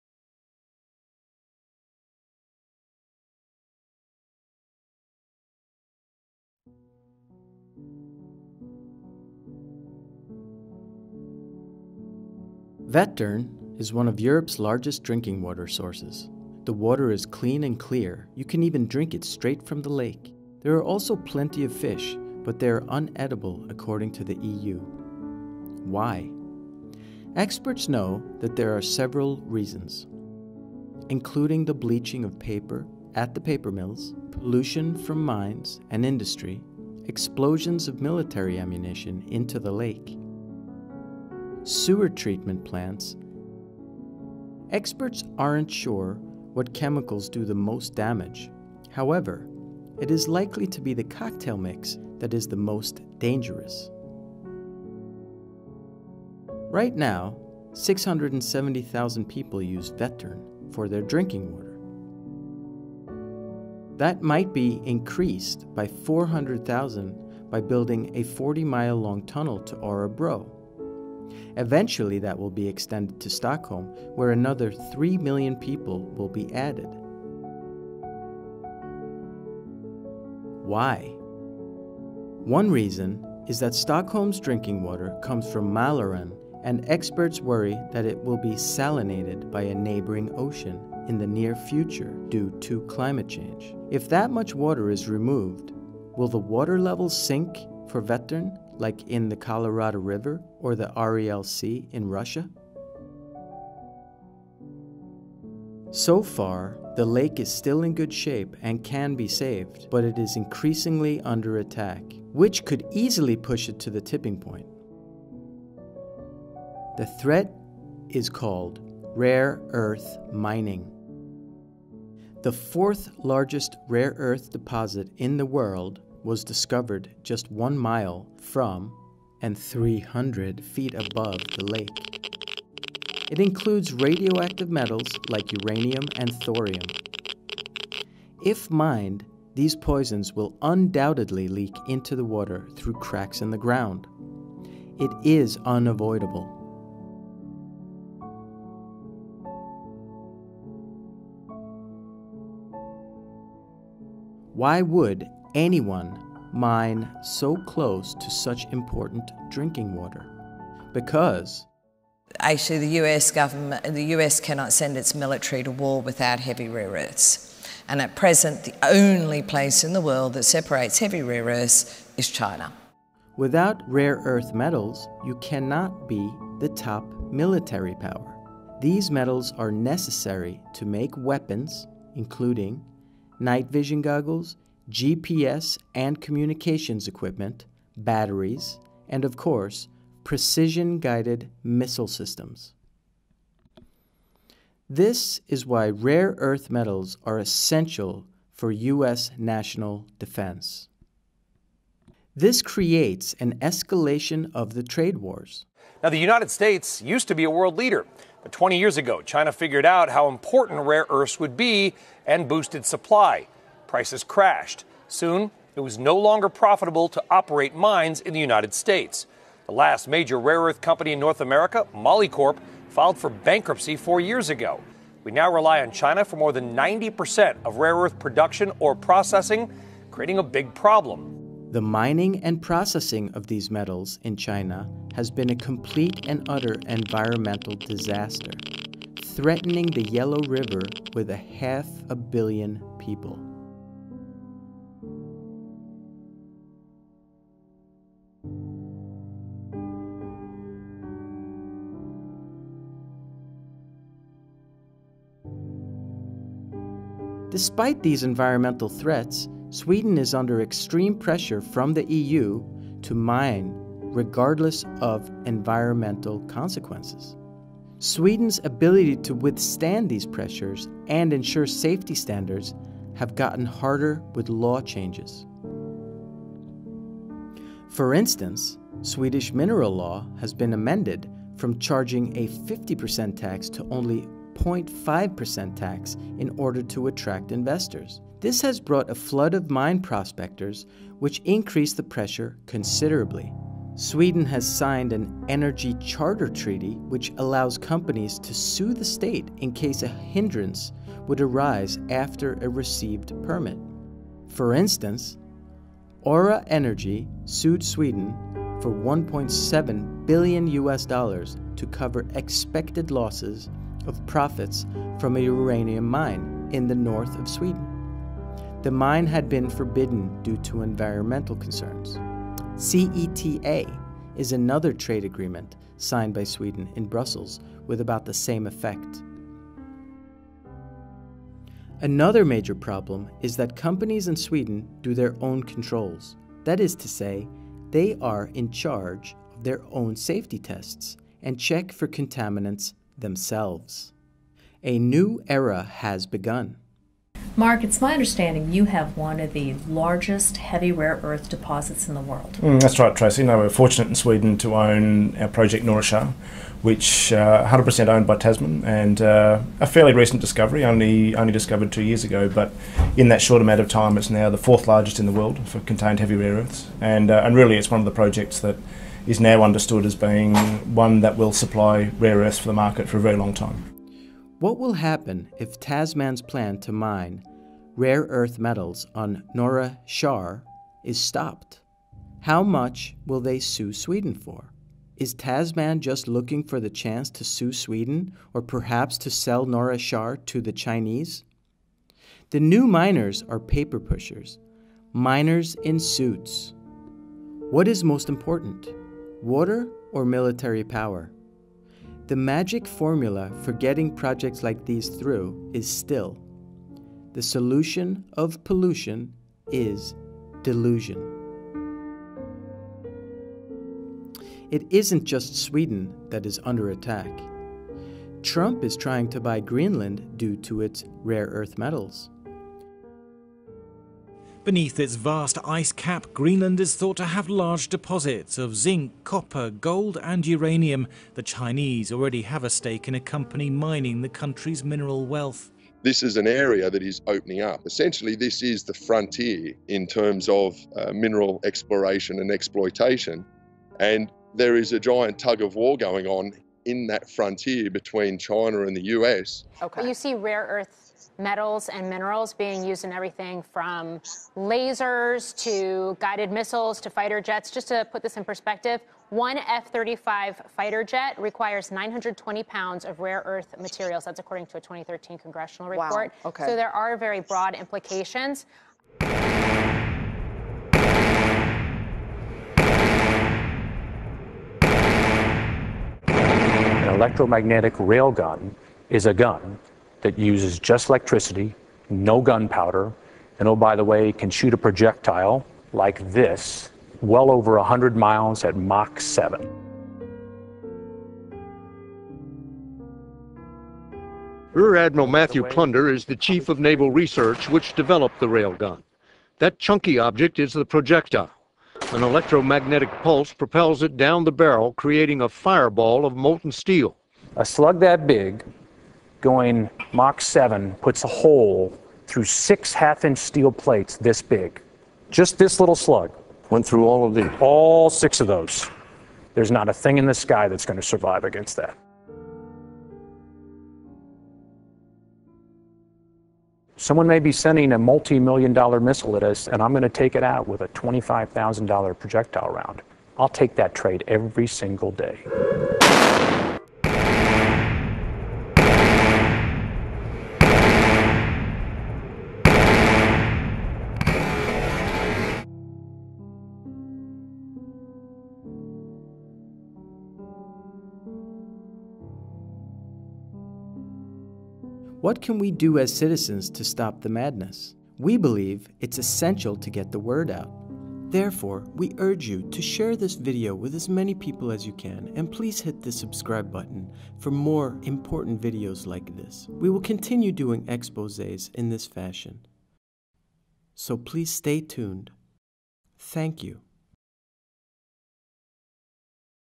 War Over Water at Vättern, Sweden.
There is a war brewing behind the curtains of big business and military might. This is no longer exclusive to third world countries anymore. It has now reached the heart of Sweden.
Swedish drinking water is at an imminent risk of being contaminated with radioactive hazards.
The EU, USA and mining companies alike are pressuring Sweden to mine a mere 1km from Lake Vättern which is a vital drinking water source.